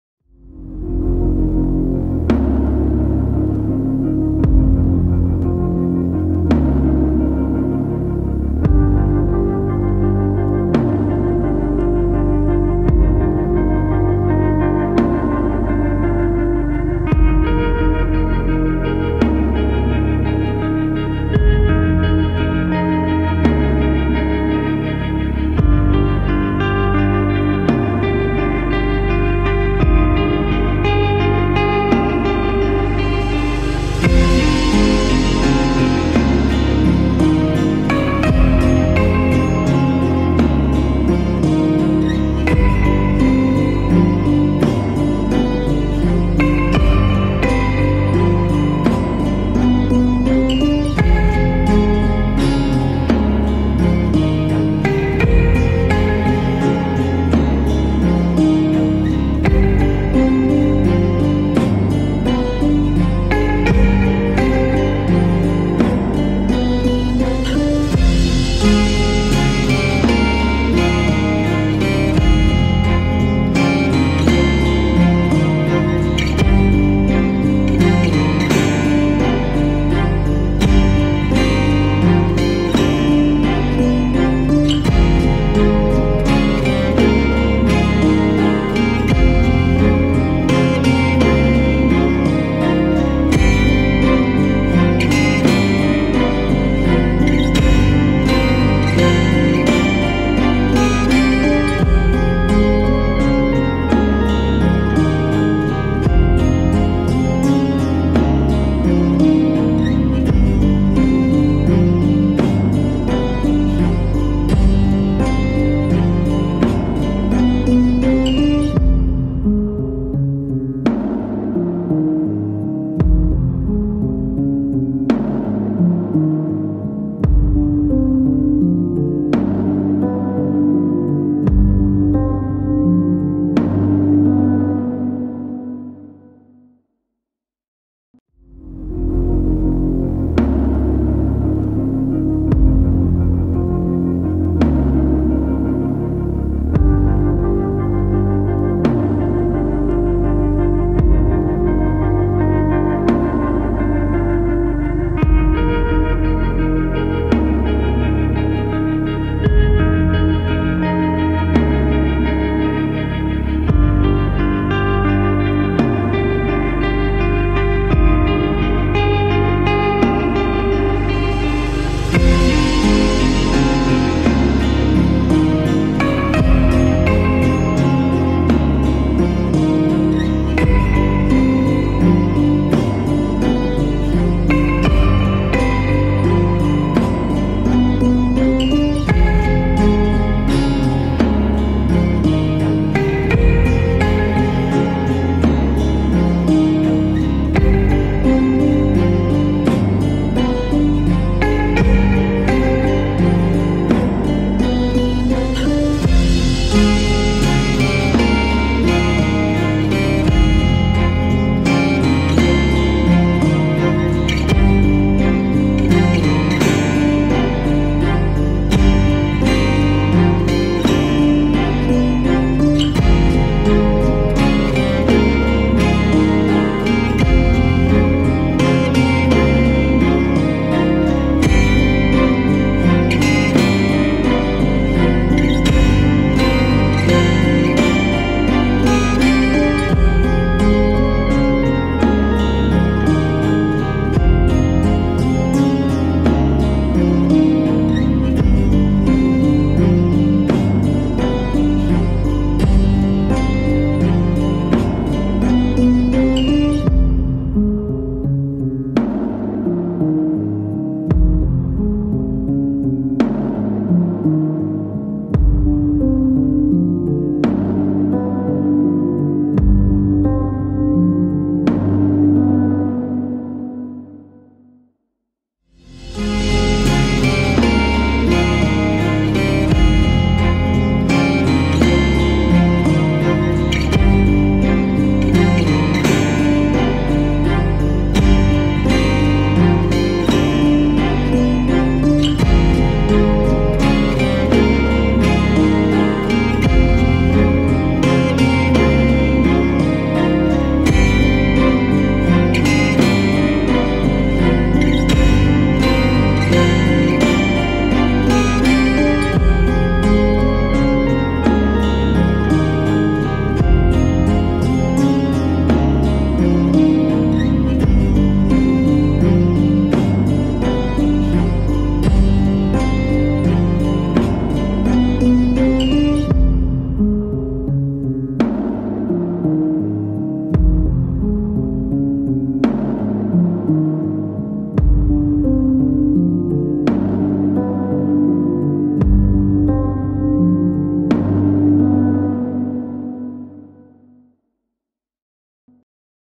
Klaus Hargreeves | Umbrella Academy
Robert Sheehan as Klaus in umbrella academy portrait
Time-lapse video